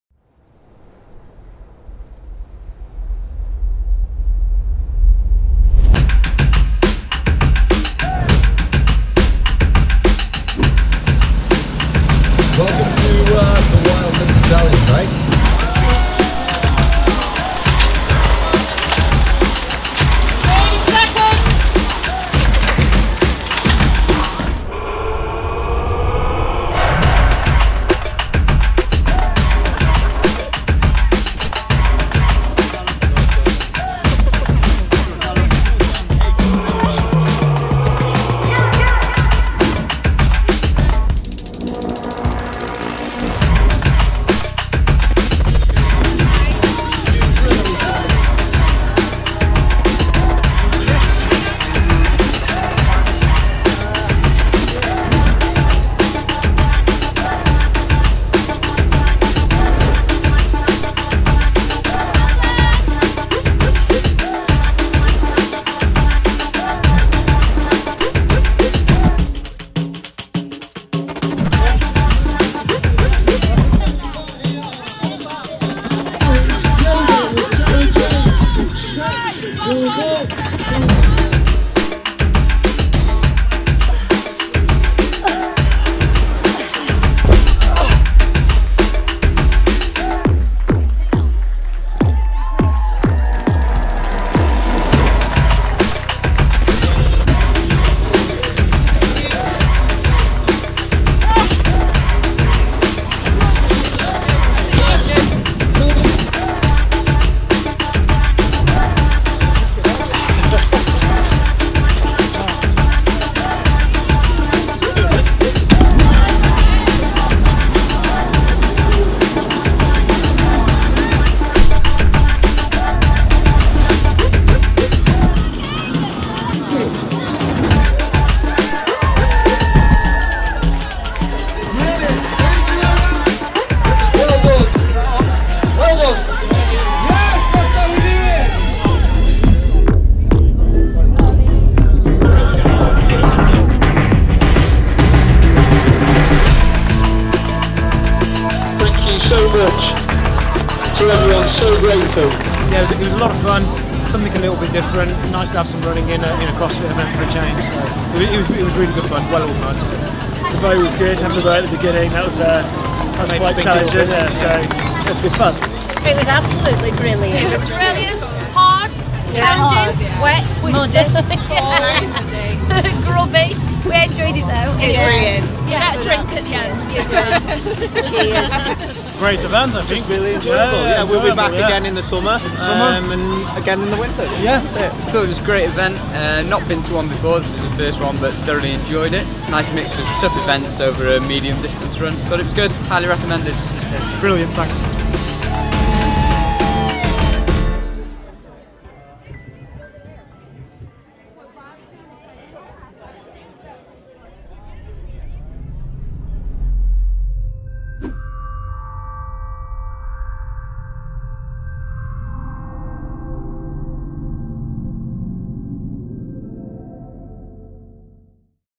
Conquer Fitness | The Wild Winter Challenge
The Wild Winter Challenge offers the ideal blend between a middle-distance run, with a test of power, output and stamina. Serving up a variety of stimulating workout stations over 6-8 kilometres of rugged terrain and all in the cold and wet weather of a dreary January.

Be prepared to run, jump, lift, row, maul, flip, climb and to take on the challenge and push you deep into your discomfort zone. Sounds grim, it will be.